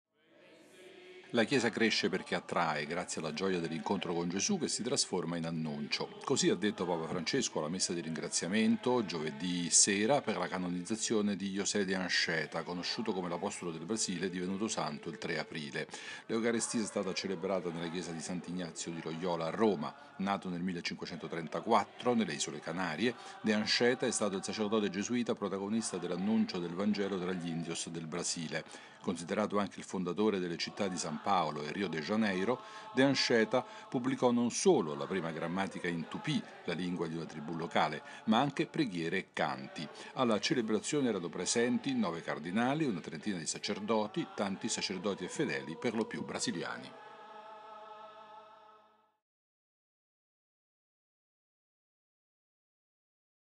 Papa Francesco Sant'Ignazio di Loyola 24-04-2014
Papa Francesco ha celebrato nella chiesa di Sant'Ignazio di Loyola, a Roma, la messa di ringraziamento per la canonizzazione di José de Anchieta, il sacerdote gesuita, nato nel 1534 nelle isole Canarie, che ha portato il Vangelo fra gli indios del Brasile.